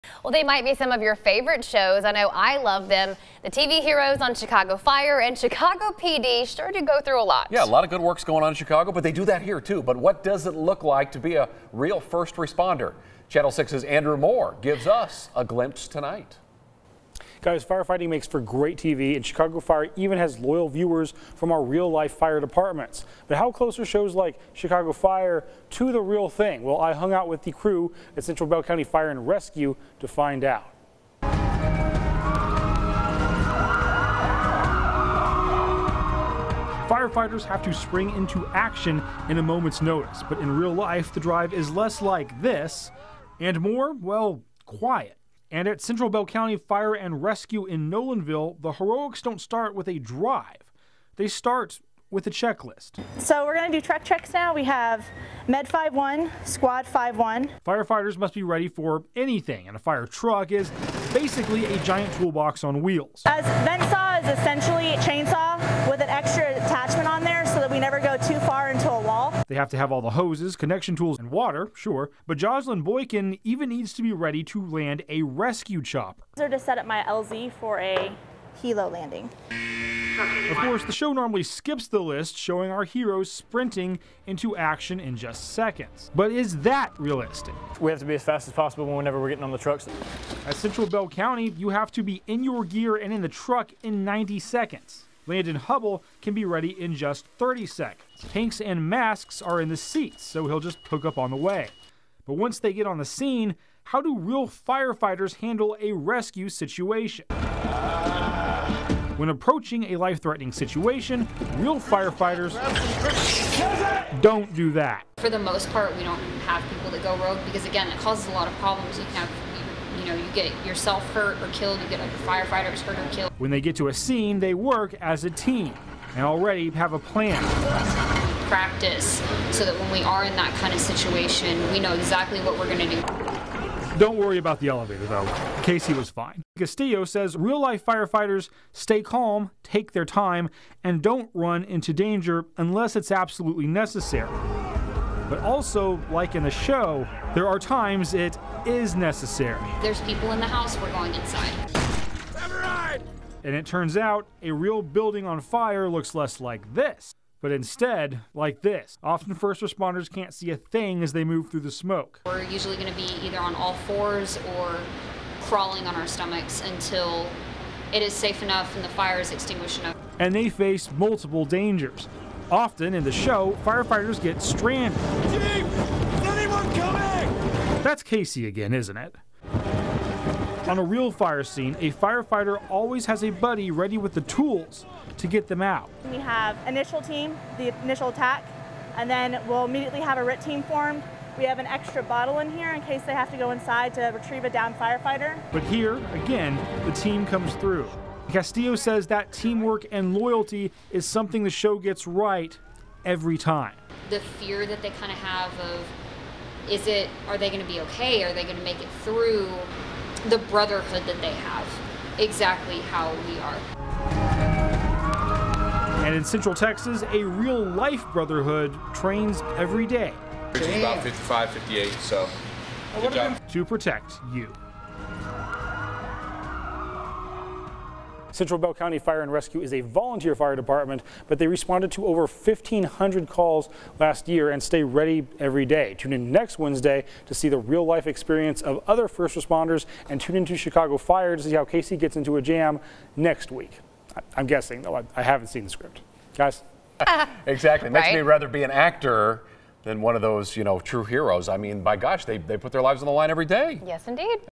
Firefighting: What's real, and what's just on TV?
Channel 6 reporter Andrew Moore rode along with Central Bell County Fire and Rescue to see how NBC's "Chicago Fire" stacks up against the real job.